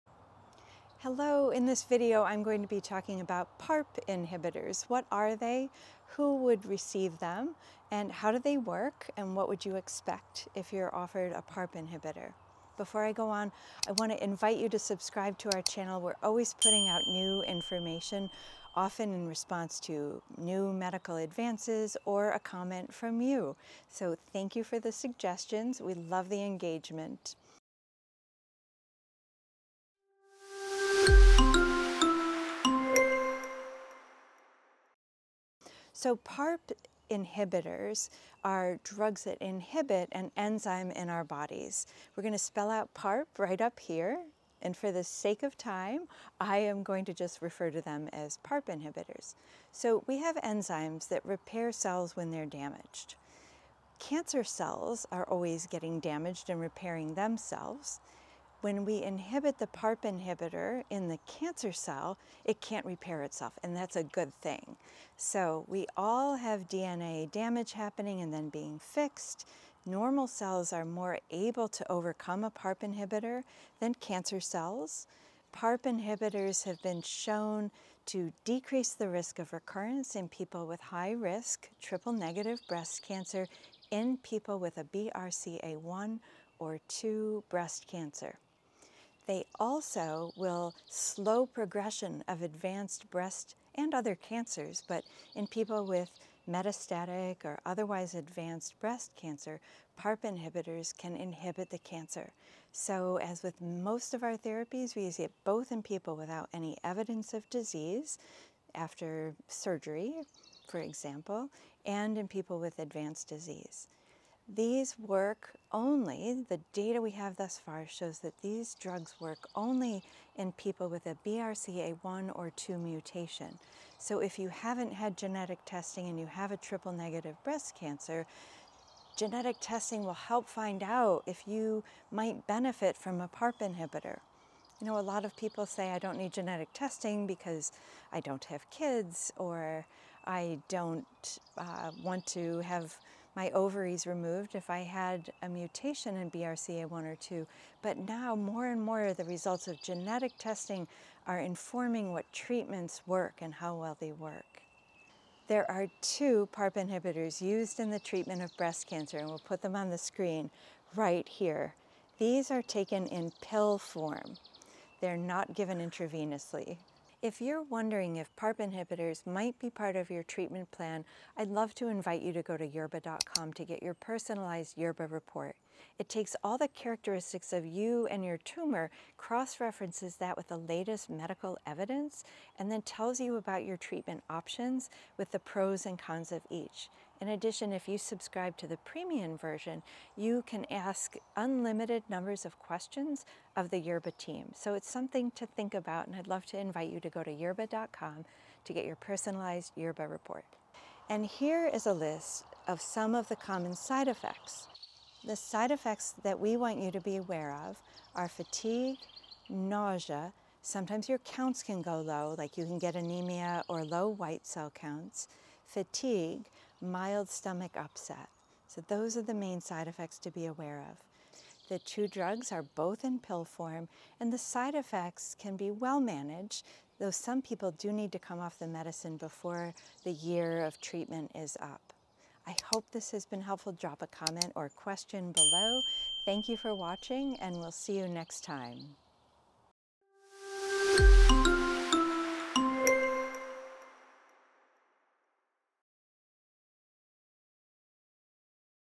What are PARP Inhibitors for Breast Cancer? | Lynparza (olaparib) Explained
Should you be taking a PARP inhibitor for breast cancer? What are the side effects? In this video, Dr. Jennifer Griggs explains what PARP inhibitors are, how they work, who is most likely to benefit from them, and common side effects. These drugs, such as Lynparza (olaparib) and Talzenna (talazoparib), are commonly used in HER2-negative breast cancers in people with a BRCA1 or BRCA2 mutation.

     Take control of your breast cancer treatment

Chapters
0:00 Introduction
0:36 How PARP Inhibitors Work
1:15 Who Gets Them? BRCA Mutations
3:21 Common Side Effects